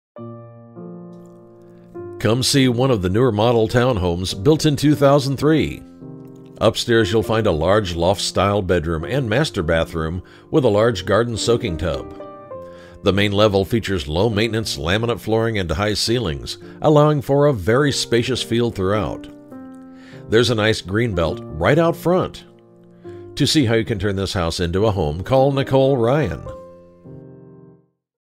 Nicole Ryan presents 140 S Moline Street Aurora, CO | ColdwellBankerHomes.com
140 S Moline Street, Aurora, CO

One of the newer model townhomes built in 2003. Upstairs you will find a large loft-style master bedroom with French doors and the master bathroom with a large garden soaking tub. The main level features low maintenance laminate flooring and high ceilings allowing for a very spacious feel throughout